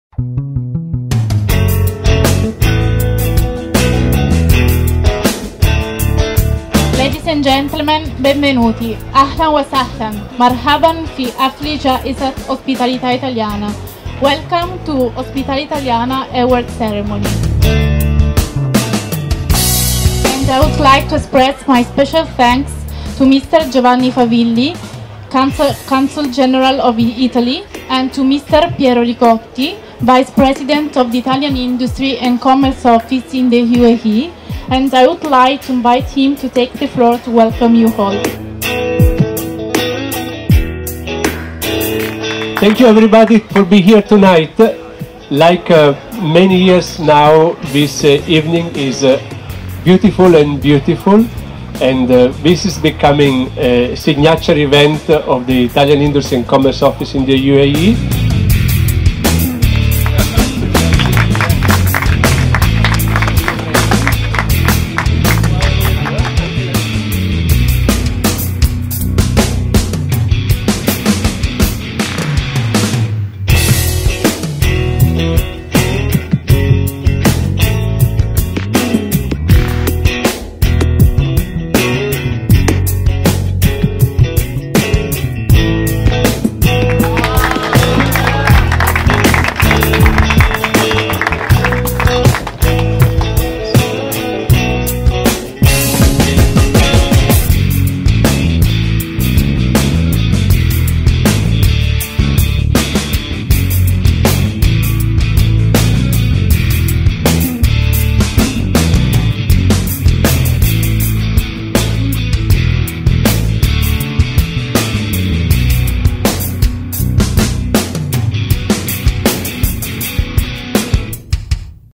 Ospitalità Italiana Award Ceremony 2015 @ Bussola
The Italian Industry & Commerce Office in the UAE is pleased to invite you to the Ospitalità Italiana Award Ceremony.

Ospitalità Italiana, Italian Restaurants in the World is a
project of the Istituto Nazionale Ricerche Turistiche (Isnart –
National Institute of Touristic Researches), together with
Unioncamere, the association of the Italian Chambers of
Commerce, aiming at recognizing, certifying and awarding the
best, authentic Italian restaurants in the world.

In the United Arab Emirates the IICUAE takes care of the
UAE-based Italian restaurants’ certications and over the
years, it has created a strong network of enogastronomic
excellences that strengthen the image of Italy in the Gulf.
Also this year, new Italian restaurants in the UAE have applied
for obtaining the certication and the IICUAE visited and
examined them.
The new Ospitalità Italiana Awards are now ready to be enlarged to the excellent winners in a breathtaking environment: the Italian restaurant Bussola, also an Ospitalità
Italiana certied excellence!